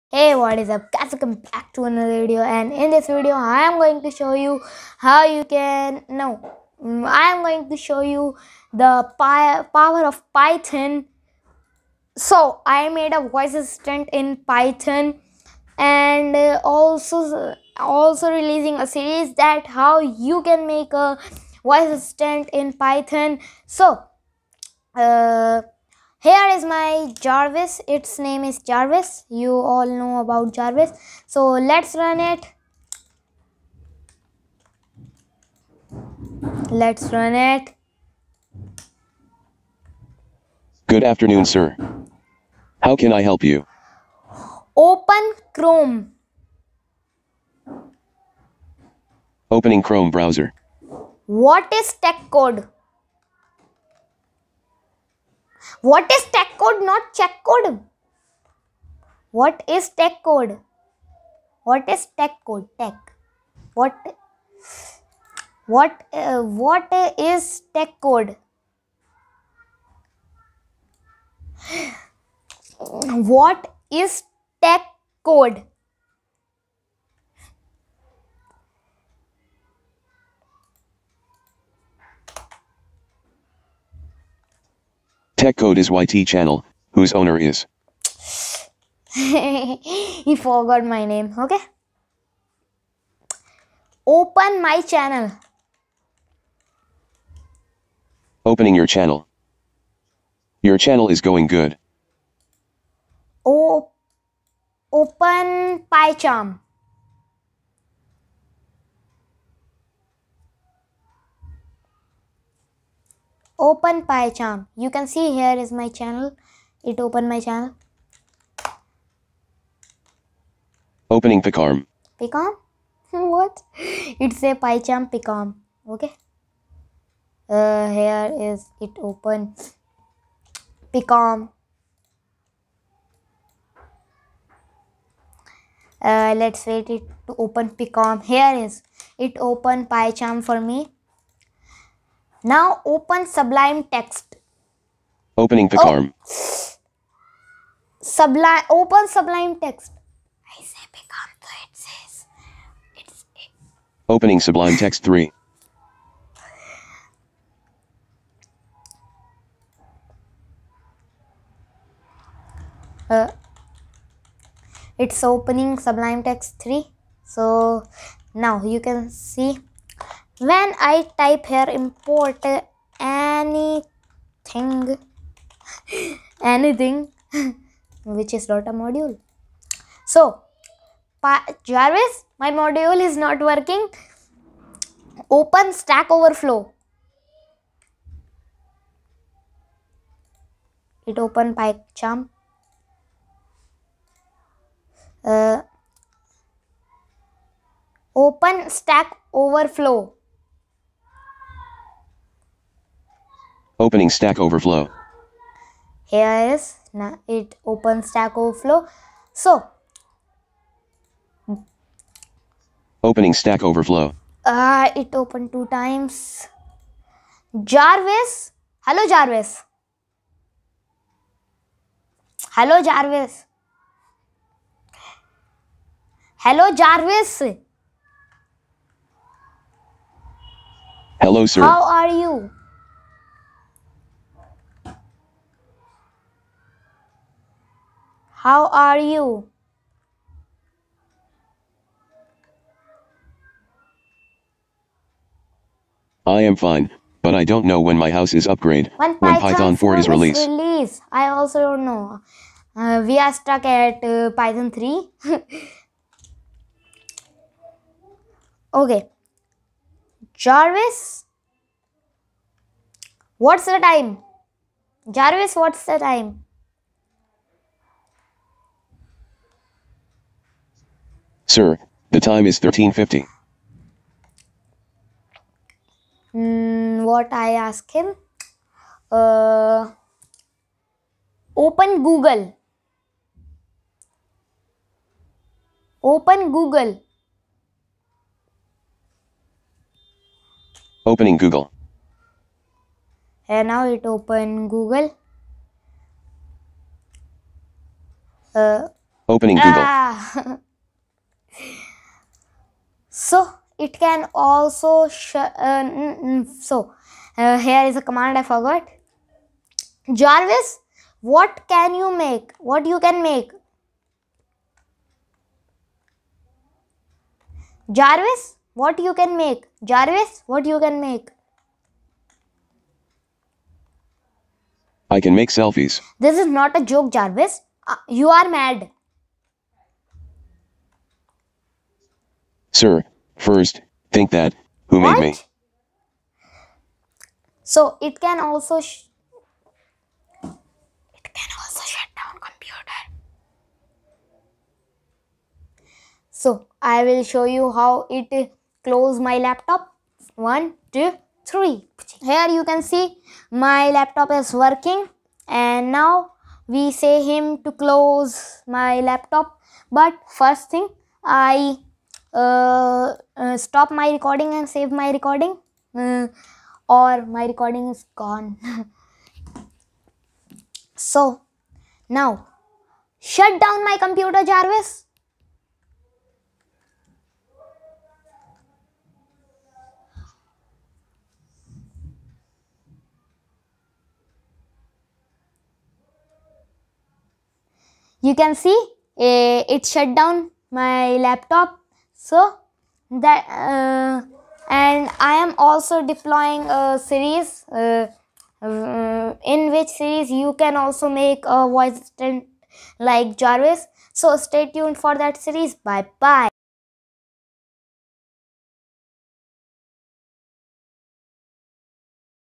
J A R V I S| AI Demo| Programming Fire
In This Video I am Going to Show You The Power Of Python
In Today video i am going to show you a voice assistant JARVIS